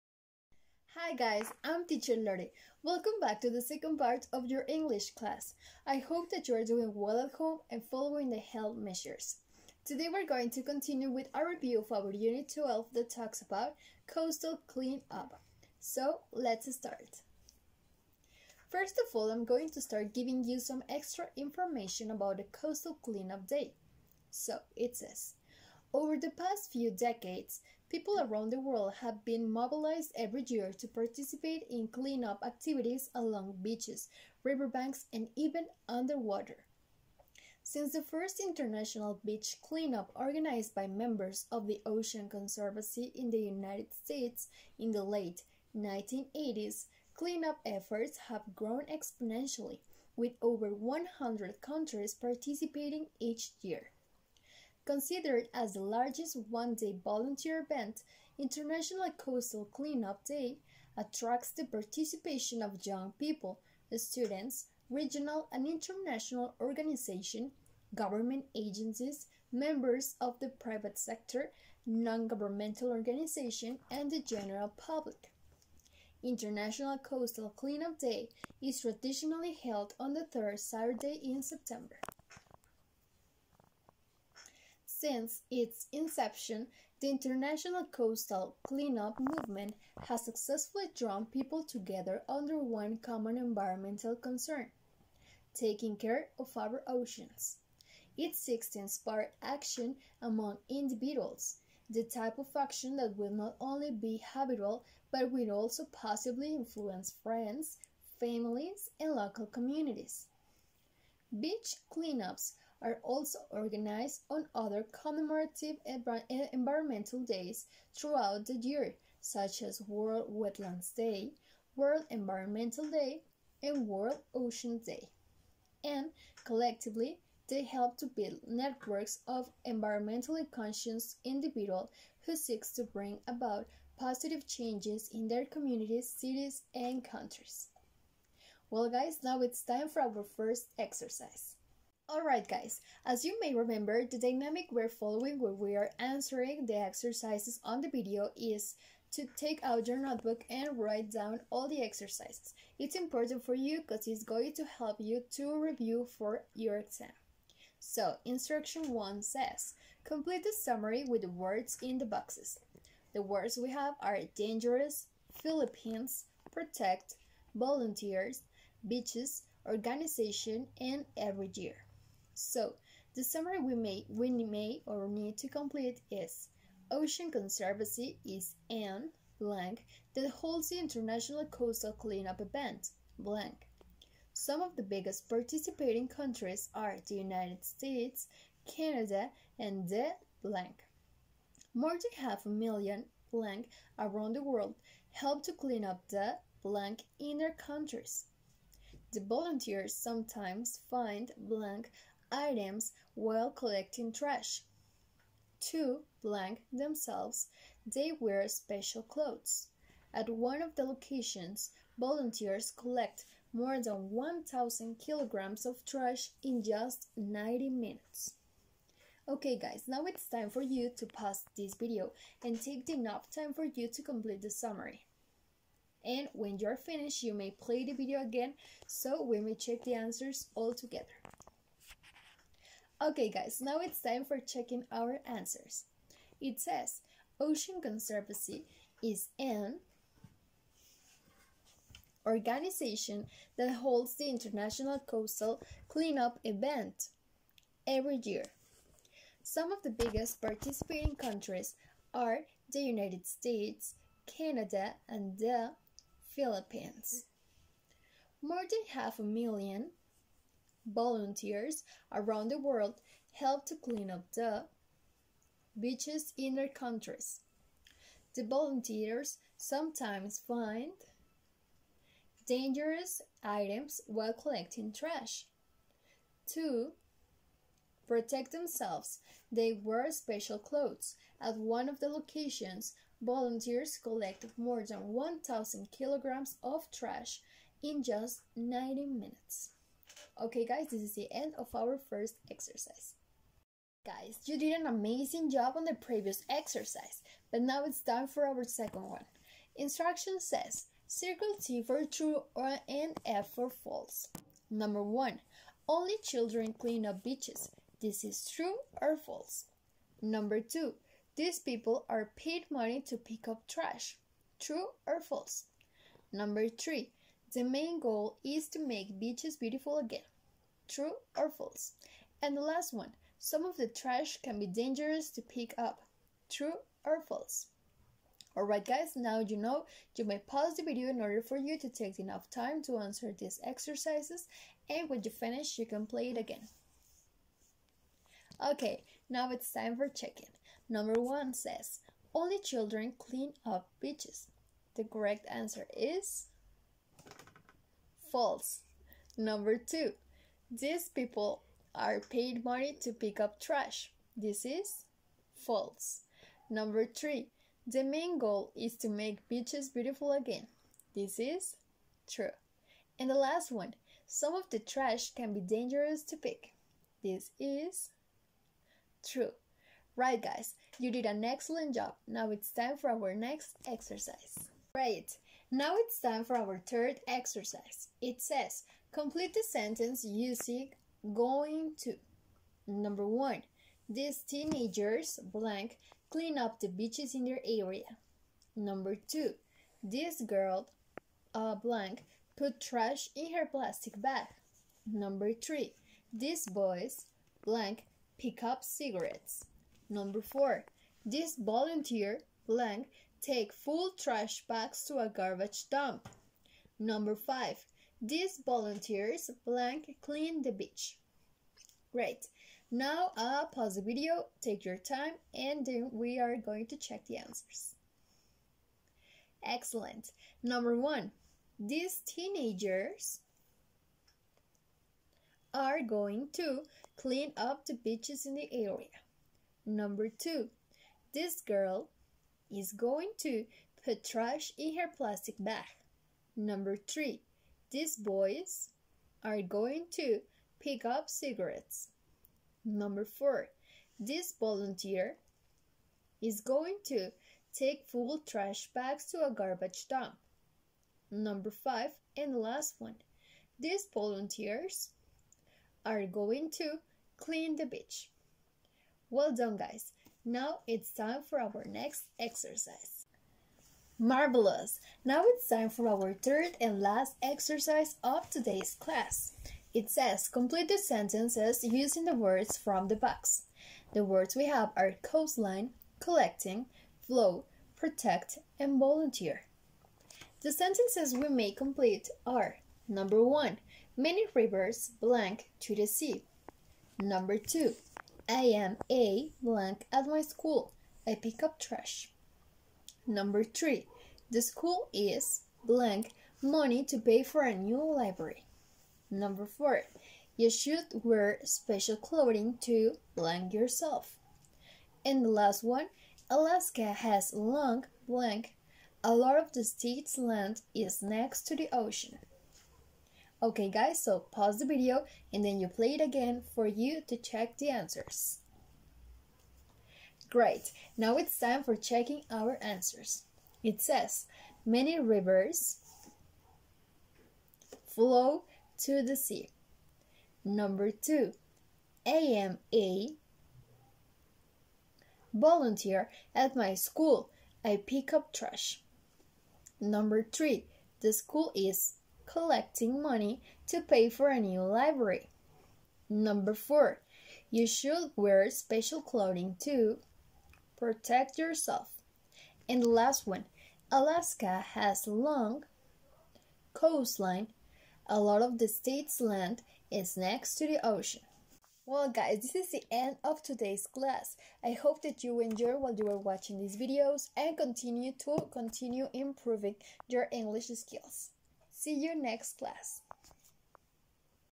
V Nivel T.Jocelyn Sesión 13 Unidad 12 Parte 2
The objective of this class (video) is to learn more information about the International Costal Cleanup Day. Also, you are going to remember some information about the video. You need your notebook.

Do not forget to review the Homework Review Session 13 video!